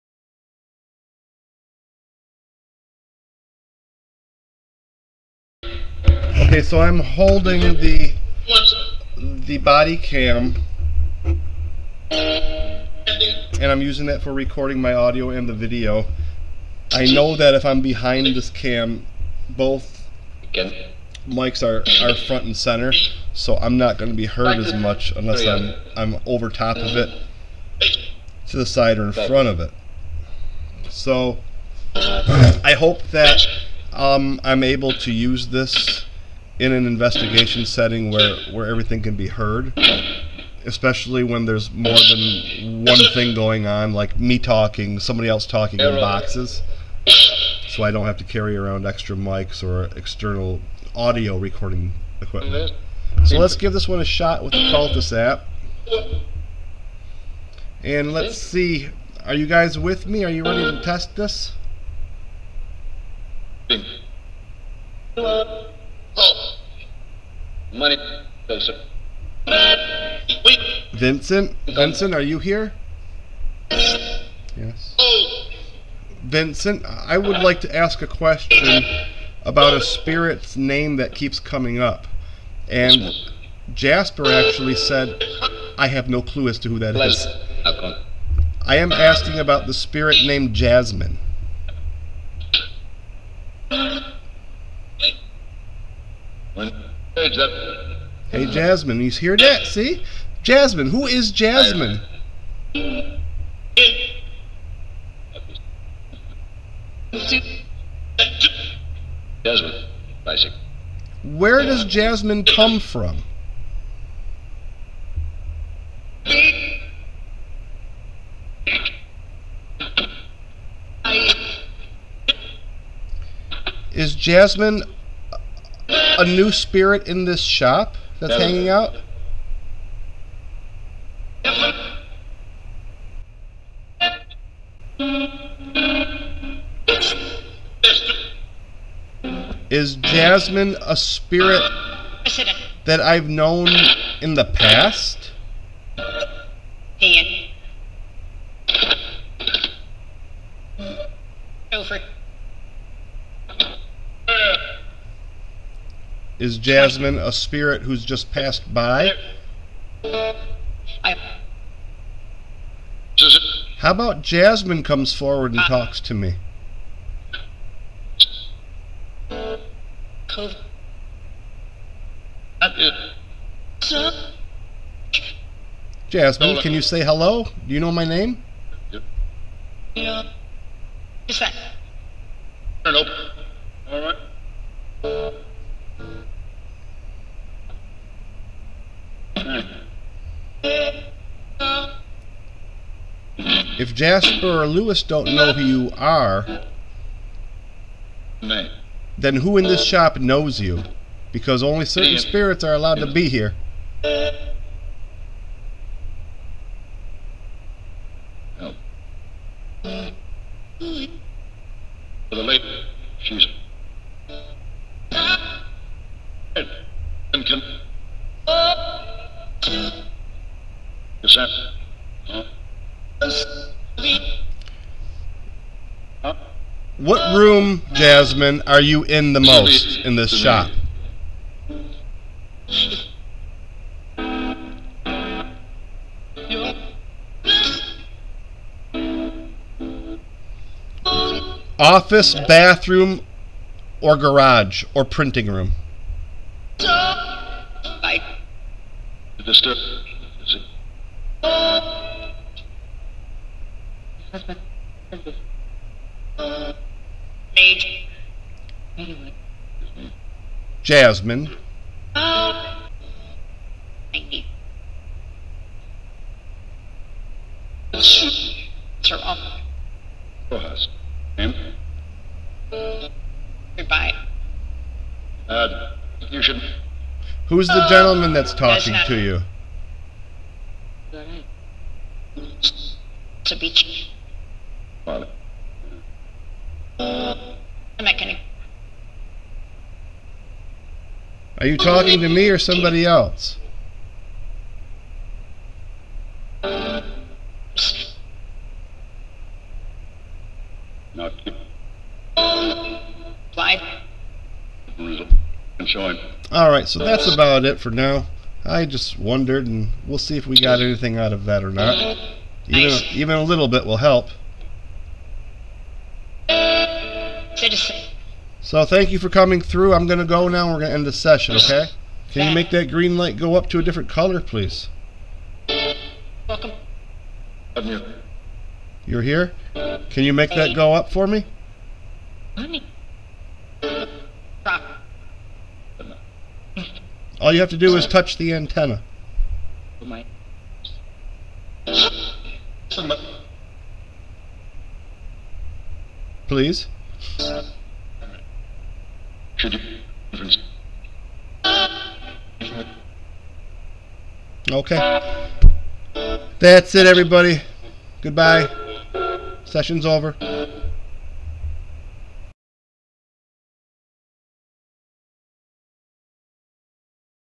Strange spirit sex shit in the shop!
This is something that has never come out in my sessions at the shop . I guess I need to find out who's there and why before I catch a sexually transmitted disease!